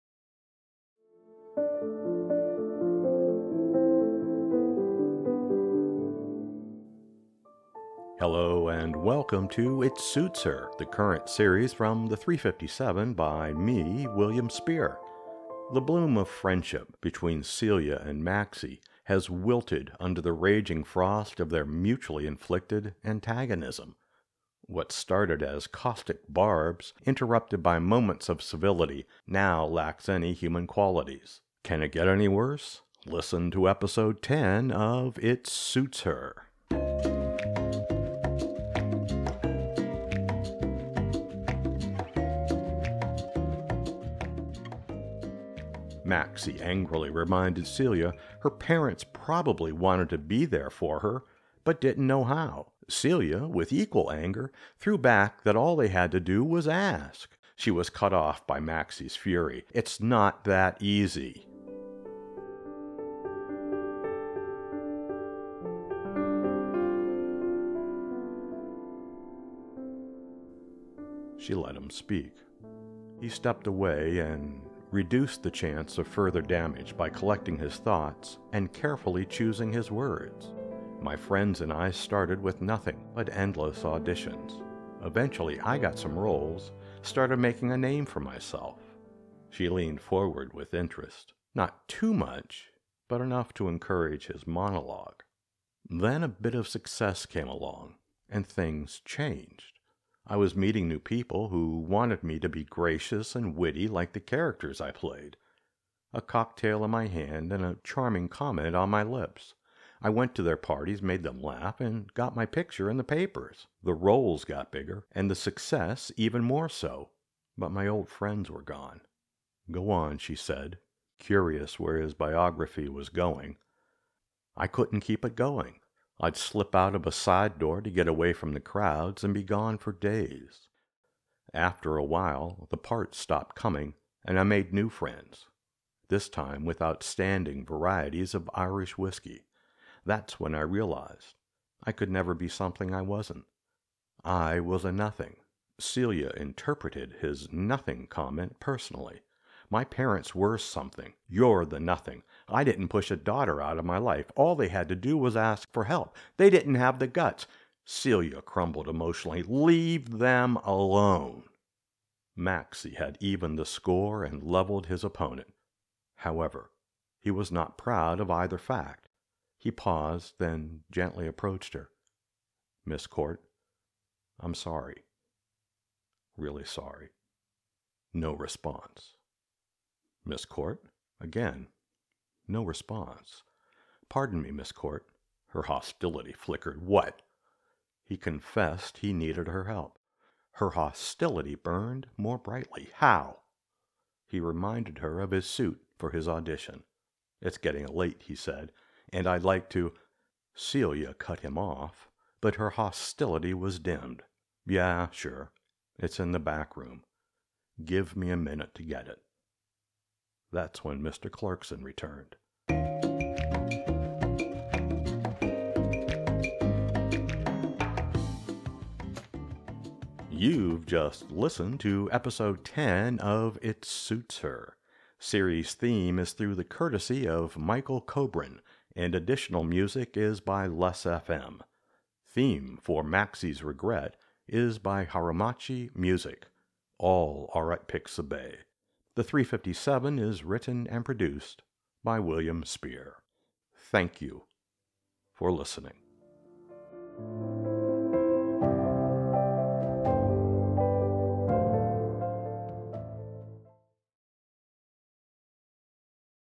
It Suits Her, Episode 10
(Theme for It Suits Her fades up and under)

Hello and welcome to IT SUITS HER . . . the current series from The 3:57 by me, William E. Spear.

The bloom of friendship between Celia and Maxie has wilted under the raging frost of their mutually-inflicted antagonism.

What started as caustic barbs interrupted by moments of civility now lacks any human qualities.

Can it get any worse? Listen to Episode Ten of . . . IT SUITS HER.

(Theme for Smart Styles Tailoring Shop fades up and under)

Maxie angrily reminded Celia her parents probably wanted to be there for her but didn’t know how.

Celia, with equal anger, threw back that all they had to do was ask!

She was cut off by Maxie's fury: "It’s not that easy!"

End of excerpt.